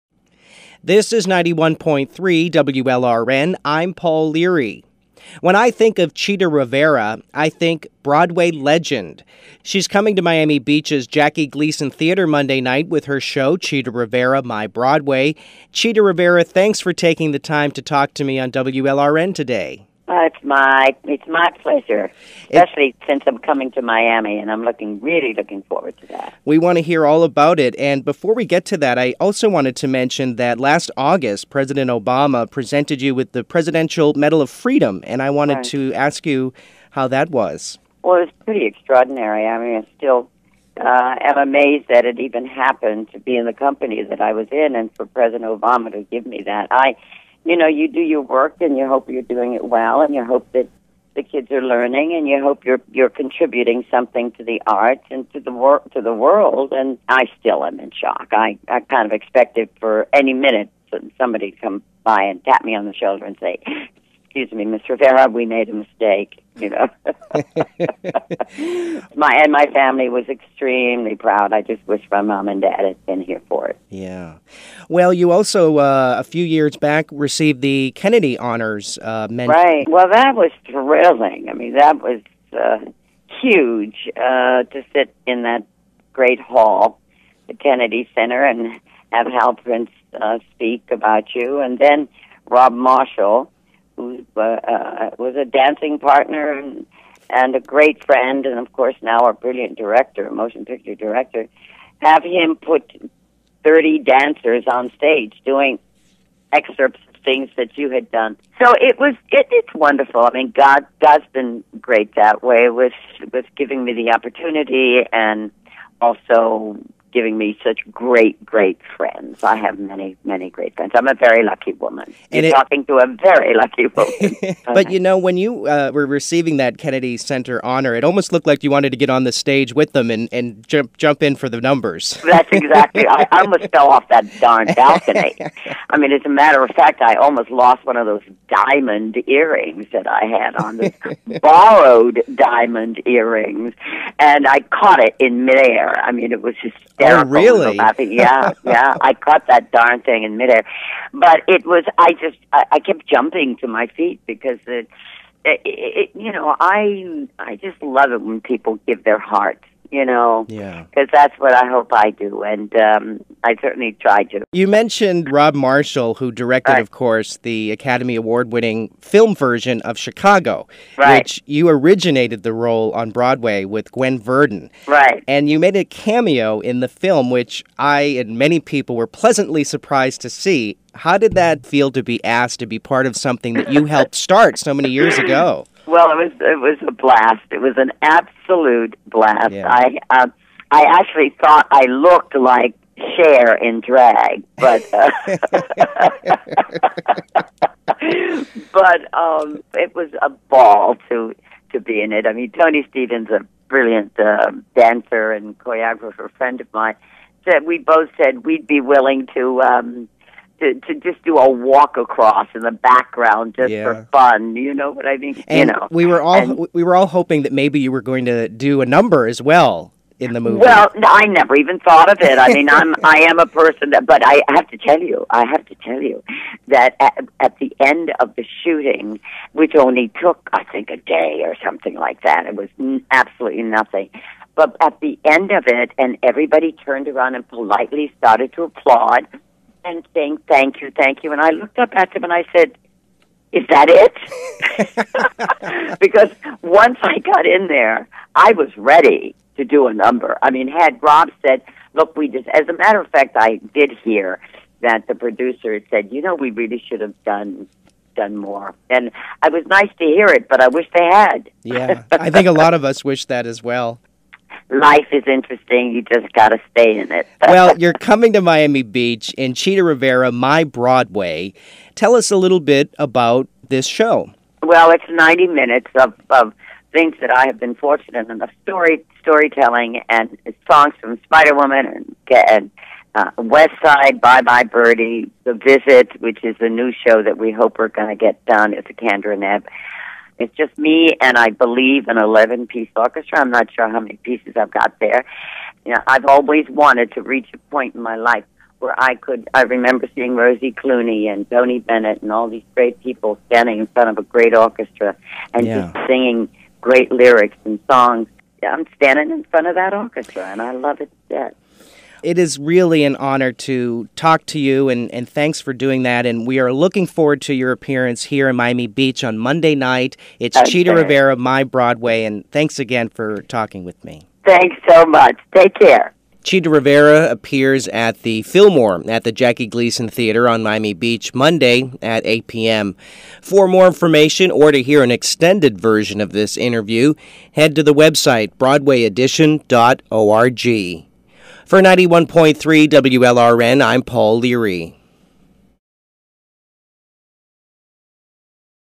Interview with Chita Rivera
Paul Leary interviews two time Tony award winning Broadway legend Chita Rivera - this is audio that was broadcast on WLRN.

Edison Farrow presents Chita Rivera: My Broadway at the Filmore Miami Beach at the Jackie Gleason Theater, Monday, April 19th, 8pm.